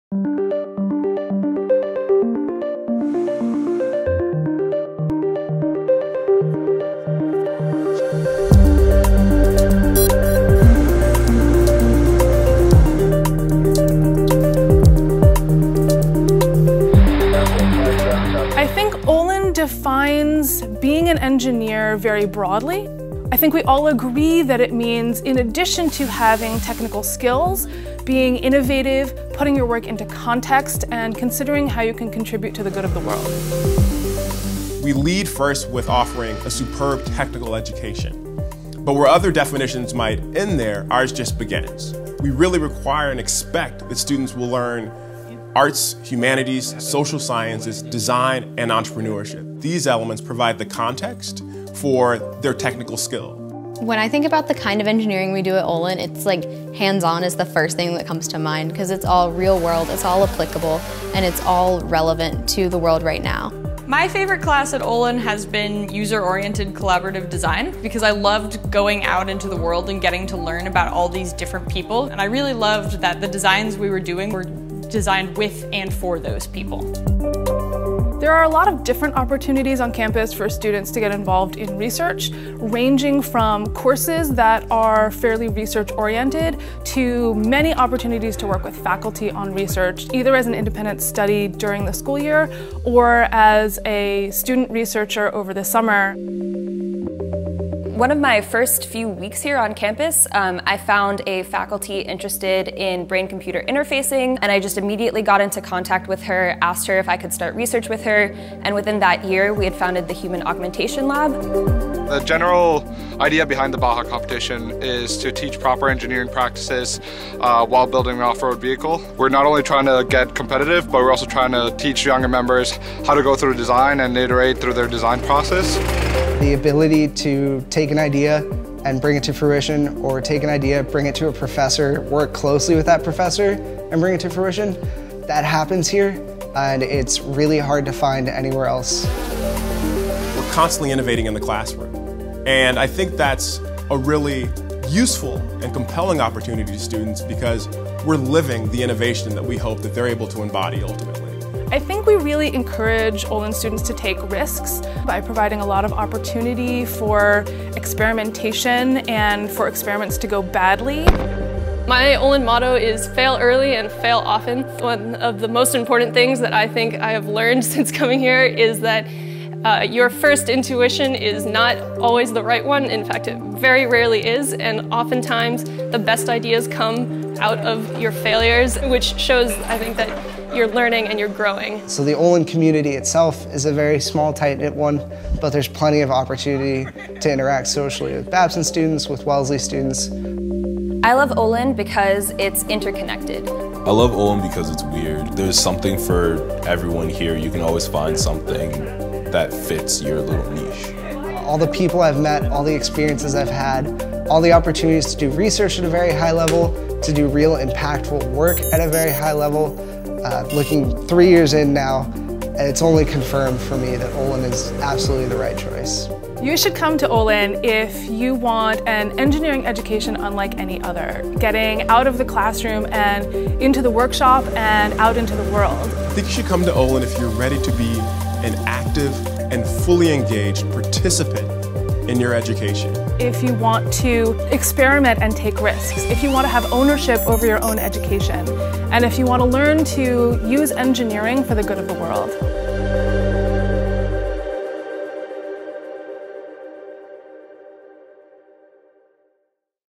Olin Admission: Students and faculty describe what it's like to be an Olin engineer.
Do you want to know what it's like to be an Olin engineer? Watch our 4-minute video to hear from students and faculty about what it's like to be at Olin and learn how to solve problems, experiment and engineer a better world.